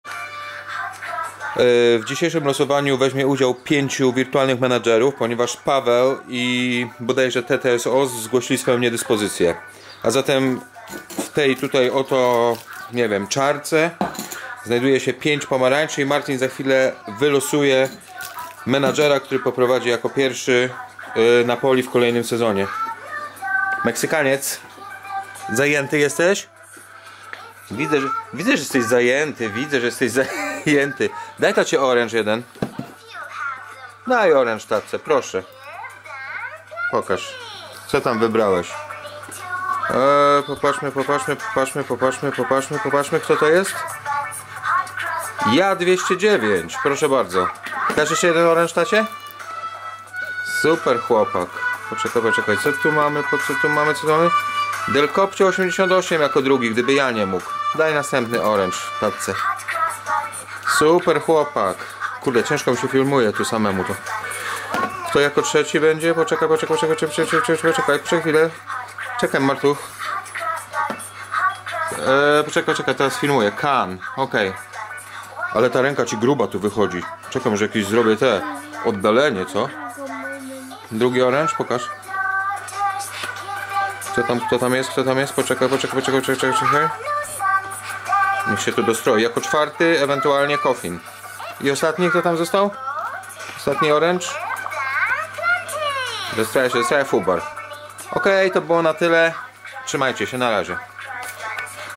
Losowanie nr 4 - Mahdi i Martin Lee.
Martin Lee losuje menedżera, który obejmie Napoli w sezonie 2020/21.